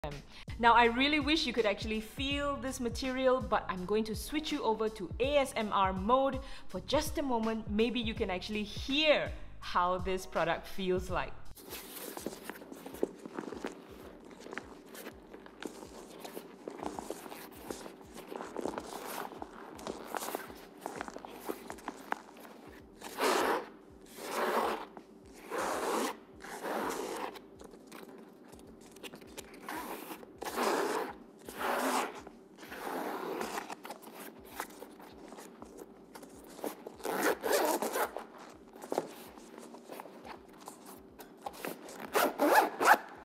ASMR with Boundary Tek Case
Since you can physically feel the amazing material on the Boundary Tek Case. Here's to hear how it feels like!

Head to our channel to watch the full product video.

If you want us to do a video on any other bags or products from Oribags.com just drop us a note at the comment section below!